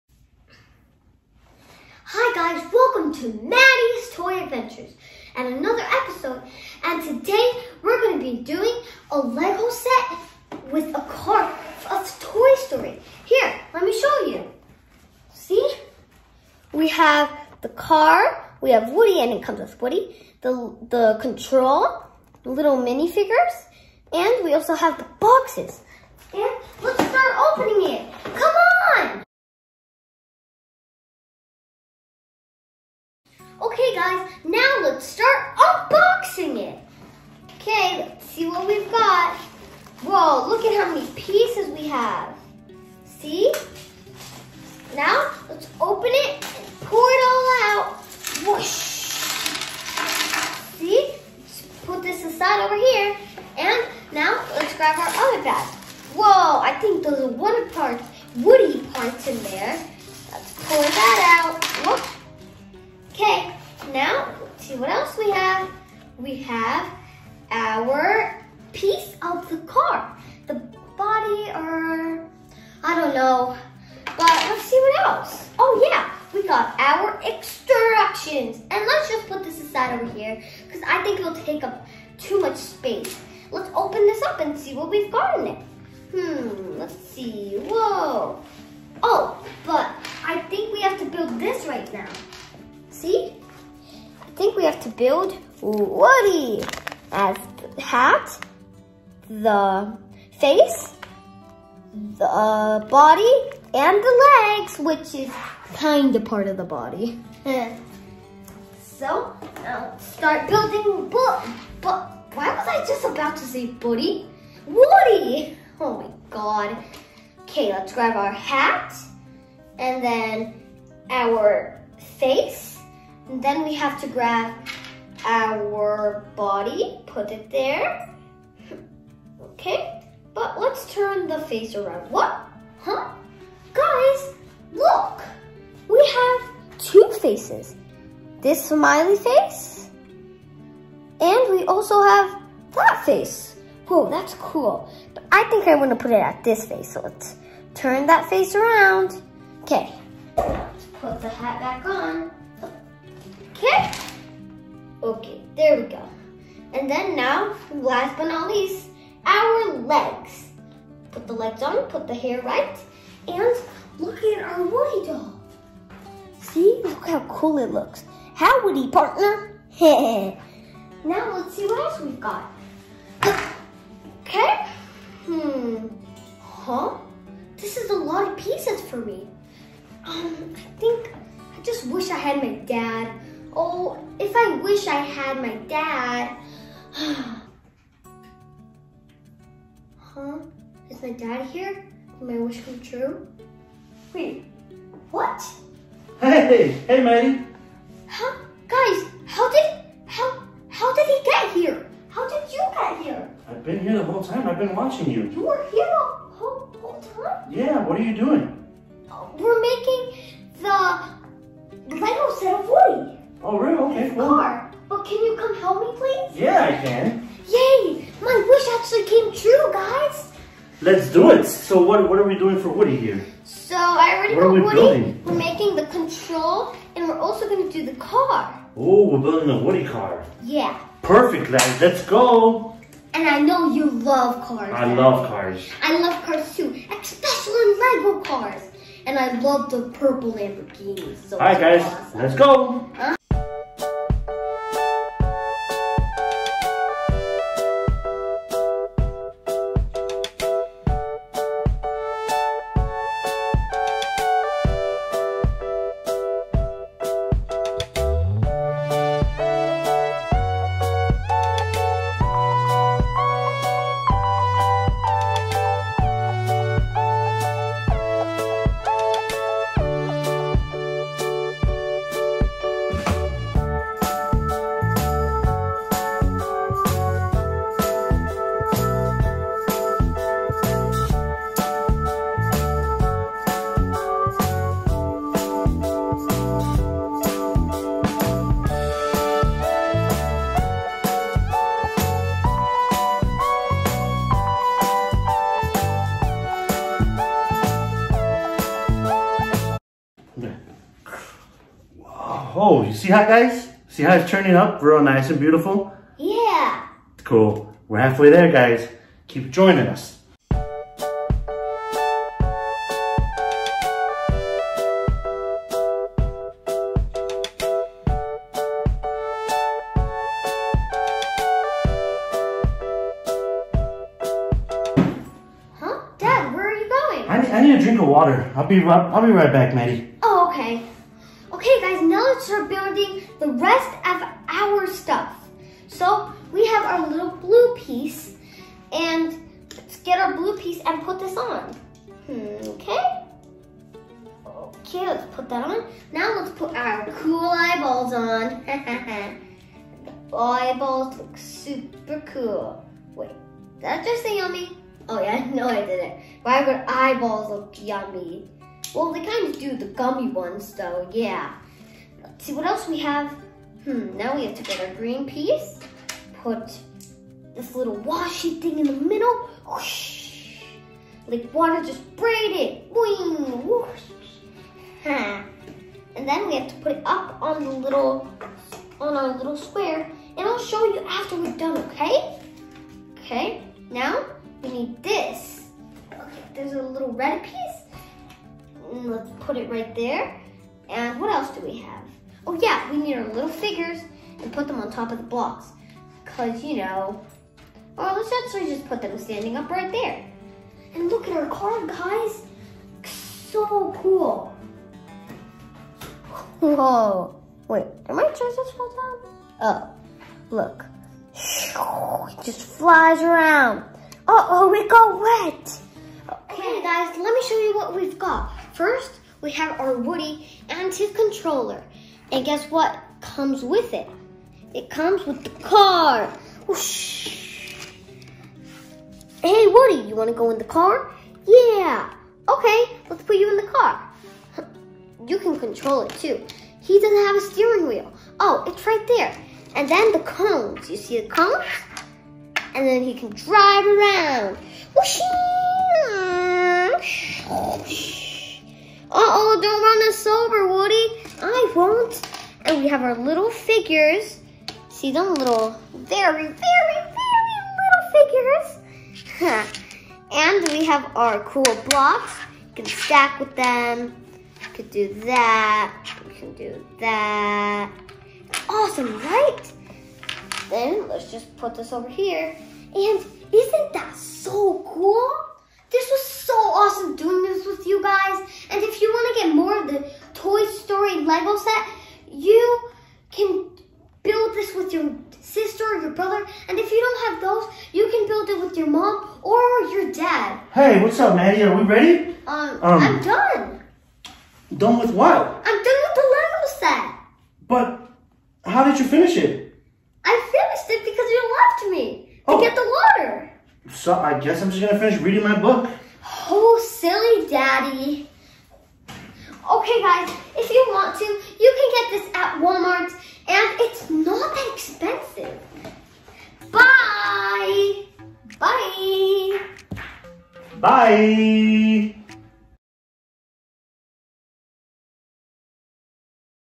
Building LEGO Toy Story Set, includes Woody
Today I get to build a Toy Story Lego set! It includes Woody and a car for him! My mom and dad bought it at Walmart!